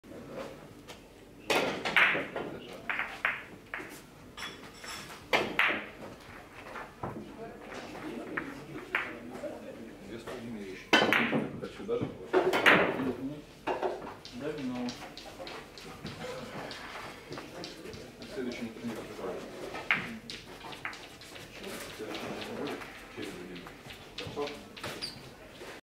Удар круазе получился вместо чужого в середину в исполнении Даниила в партии по свободной пирамиде
Удар круазе получился вместо чужого в середину в исполнении Даниила в партии по свободной пирамиде, русский бильярд.
Взявший в руки кий сразу поймет, что бильярд – это не просто перекатывание шаров, а необыкновенно красивая, умная игра, помогающая человеку раскрыть его возможности.
Круазе, или оборотный удар, – одна из разновидностей дуплета. Прицельный шар отражается от борта и пересекает траекторию движения битка.